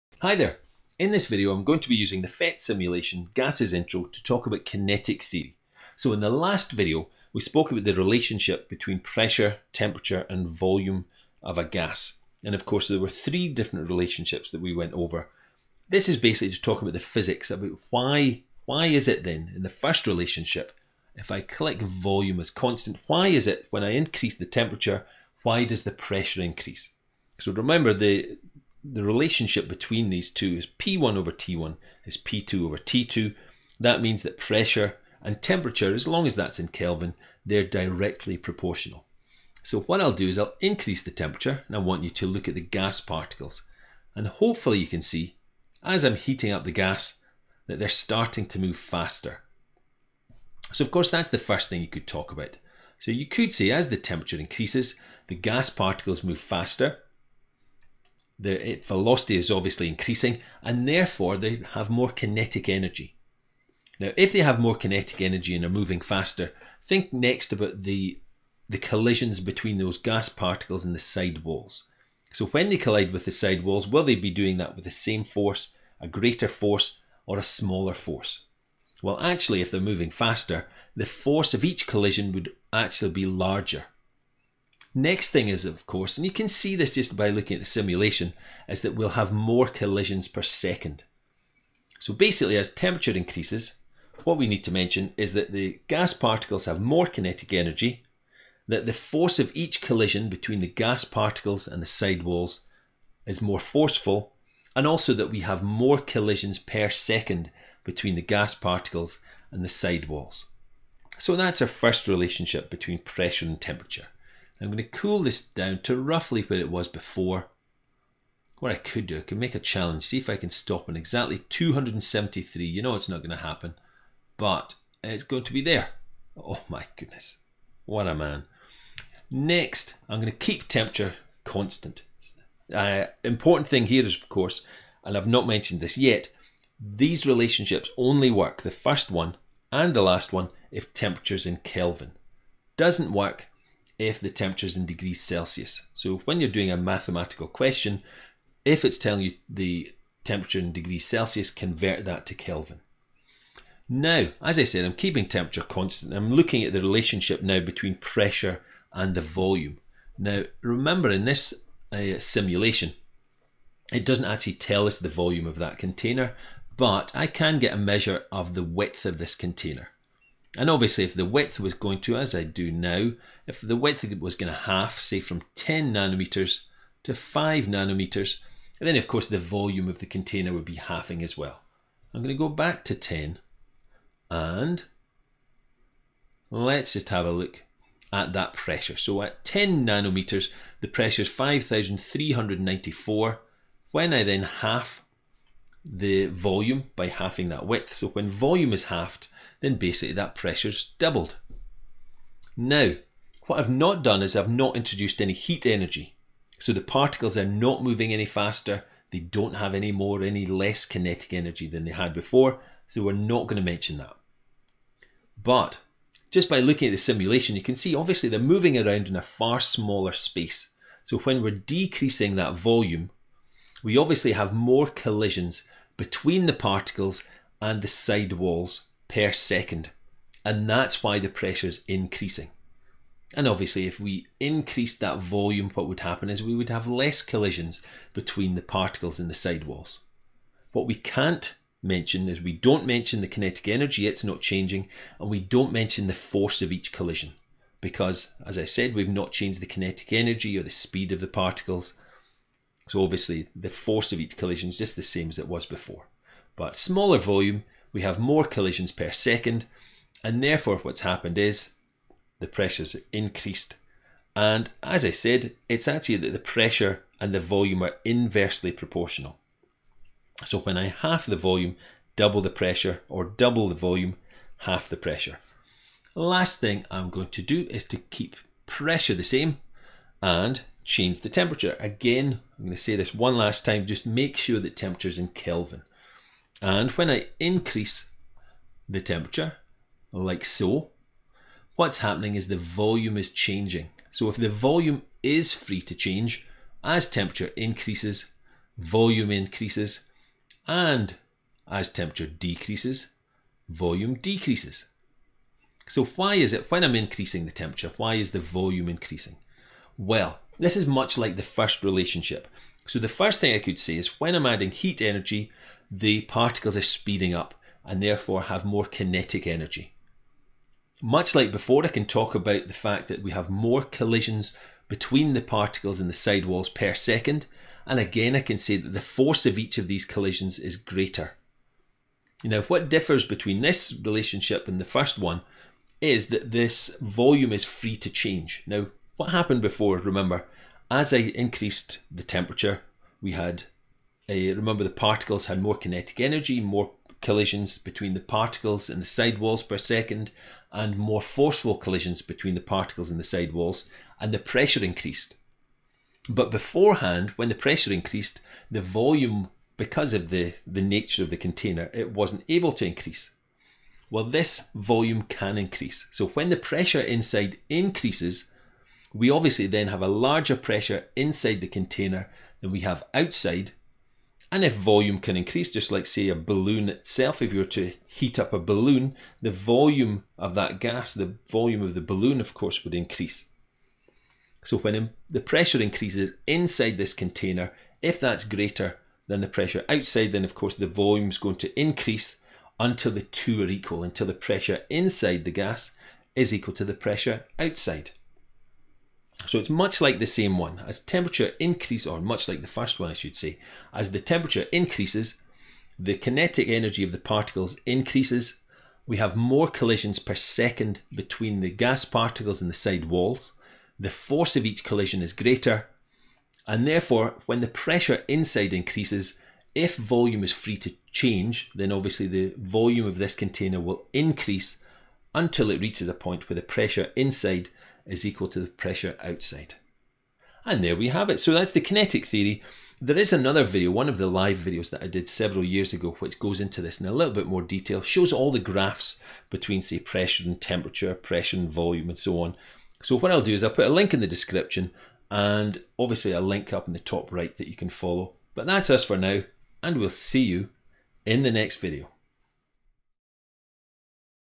National 5 Physics - the gas laws (kinetic theory) simulation
National 5 Physics - explaining the gas laws using kinetic theory. In this video, I use the PhET simulation 'Gases Intro' which can be found at phet.colorado.edu